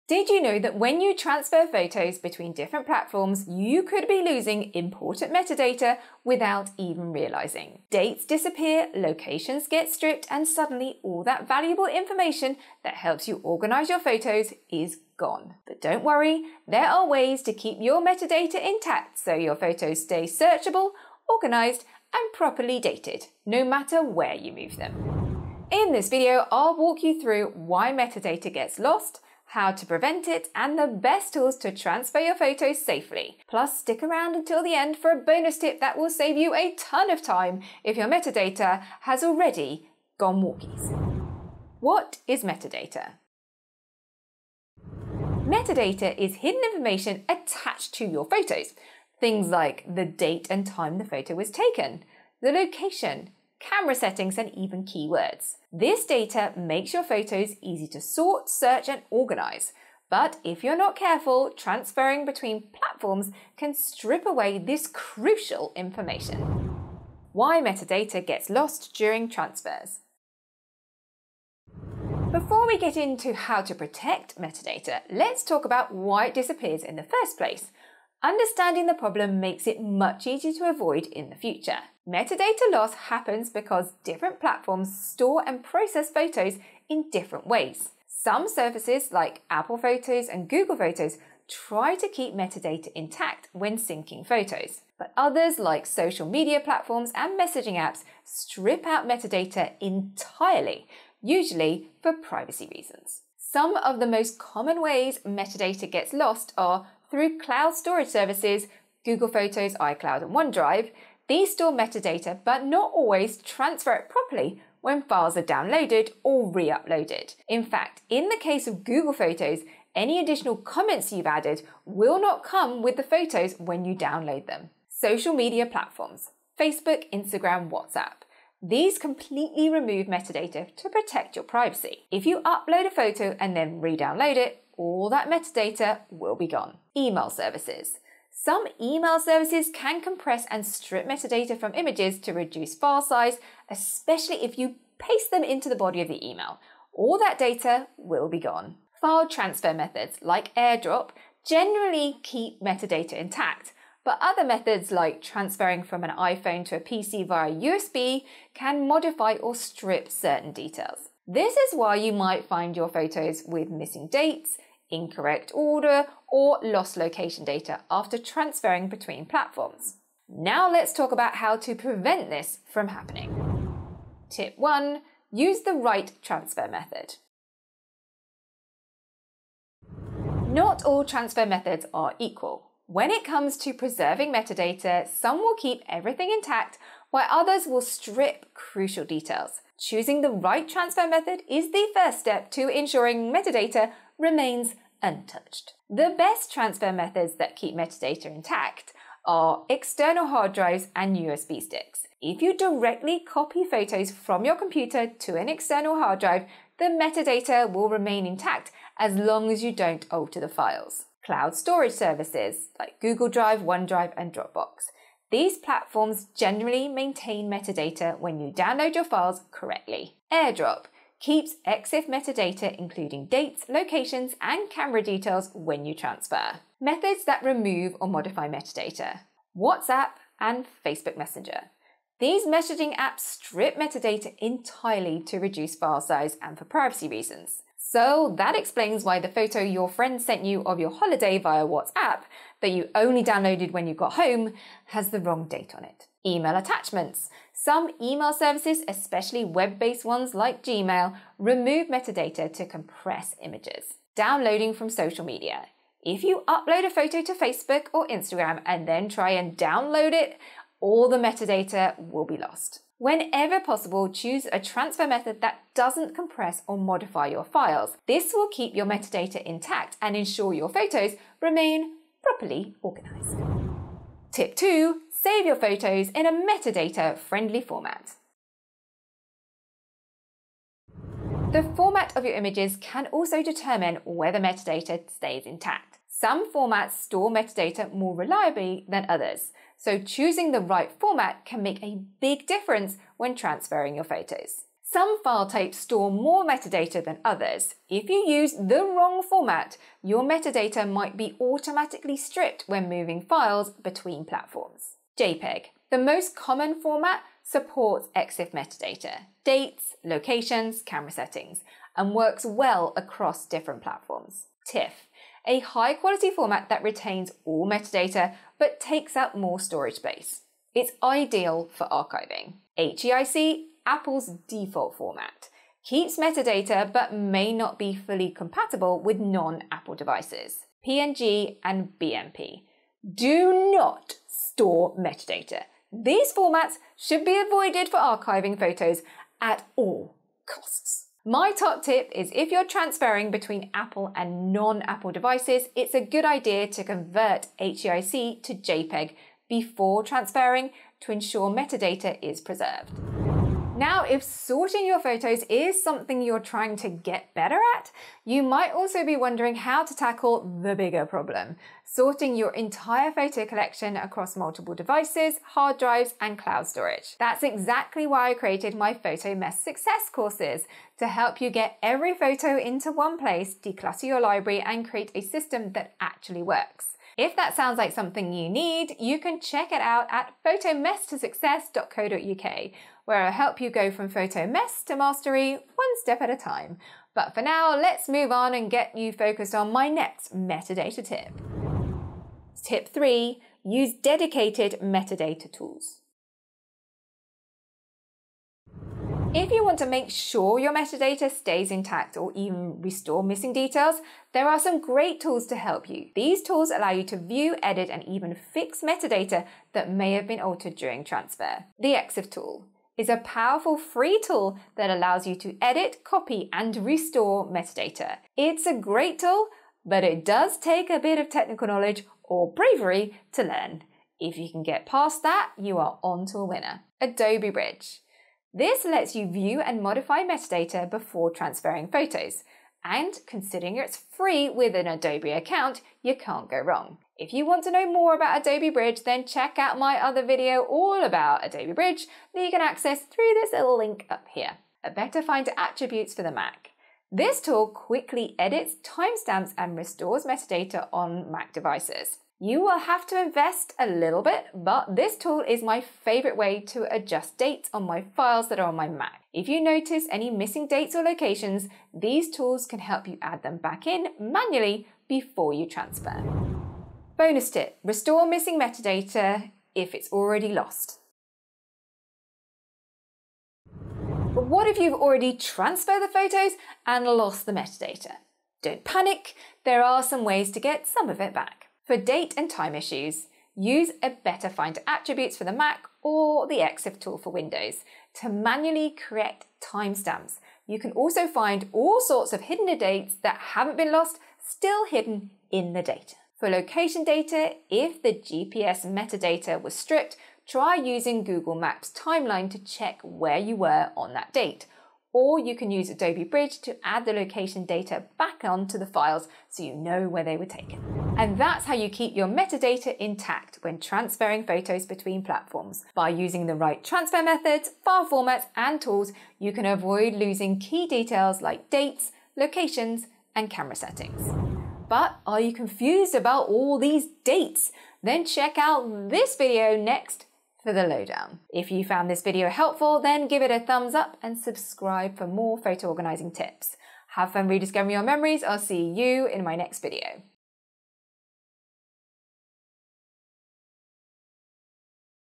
How to Preserve Your Photo Metadata When Transferring Between Platforms
Did you know that when you transfer photos between different platforms, you could be losing important metadata without even realising it? Dates disappear, locations get stripped, and suddenly, all that valuable info that helps you organise your photos is gone!
But don’t worry—there are ways to keep your metadata intact so your photos stay searchable, organised, and properly dated no matter where you move them. In this video Amanda Littlecott The Photo Organiser walks you through why metadata gets lost, how to prevent it, and the best tools to transfer your photos safely

CHAPTERS
00:00 - Intro
00:42 - What is Metadata?
01:08 - Why Metadata Gets Lost During Transfers
02:54 - Tip 1: Use the Right Transfer Method
04:35 - Tip 2: Save Photos in a Metadata-Friendly Format
06:33 - Tip 3: Use Dedicated Metadata Tools
07:48 - Bonus Tip: Restore Missing Metadata If It’s Already Lost
08:49 - What Next…

⚙️ TOOLS & SERVICES I CURRENTLY USE & RECOMMEND ⚙️
DUPLICATE FINDING